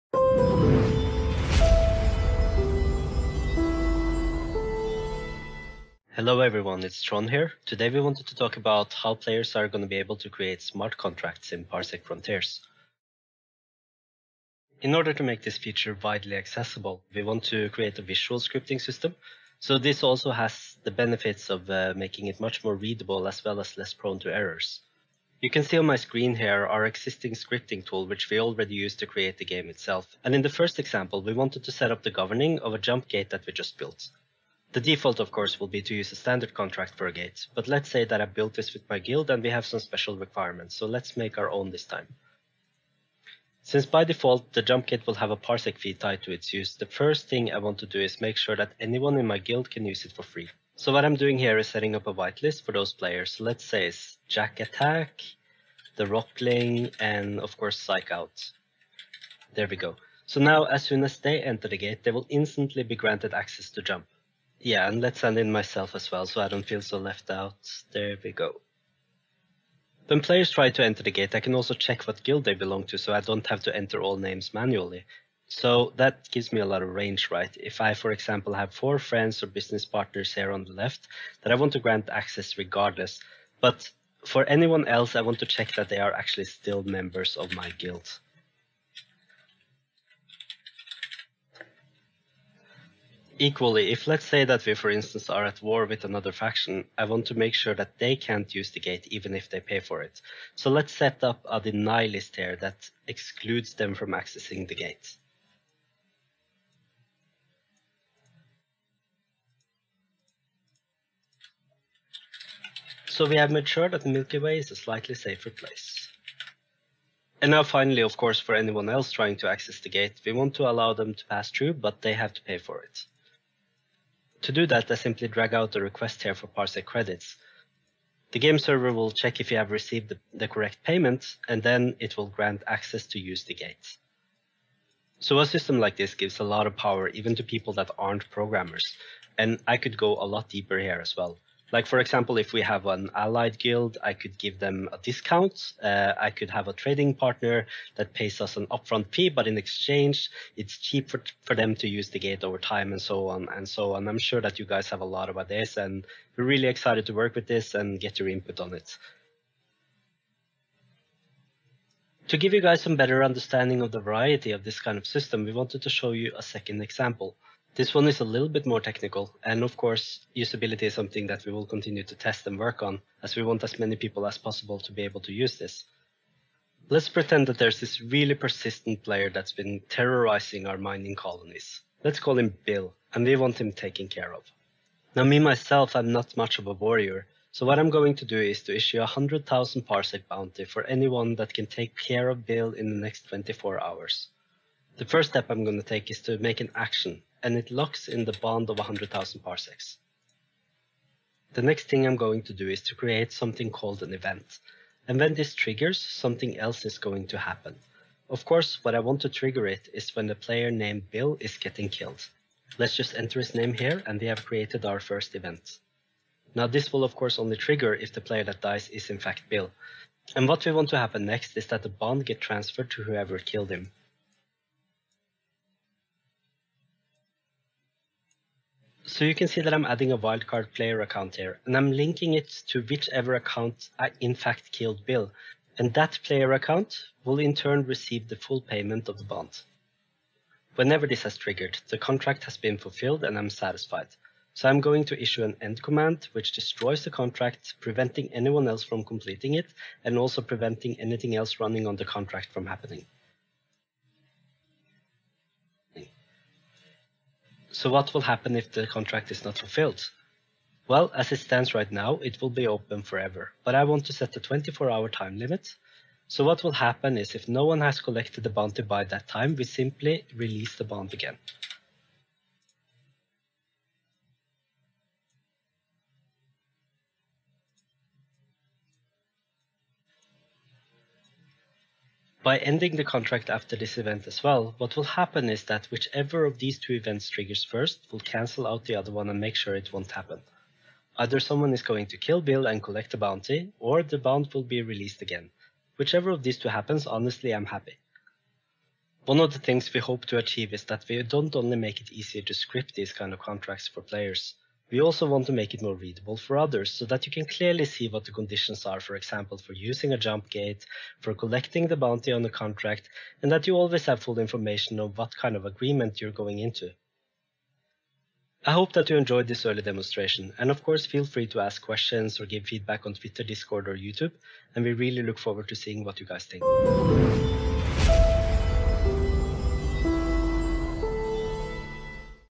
Visual scripting of smart contracts with NEO - Parsec Frontiers
Demonstrating the visual interface for simple and user-friendly creation of smart contracts for the NEO blockchain platform by the Parsec Frontiers team.

for more information about our virtual galaxy blockchain project.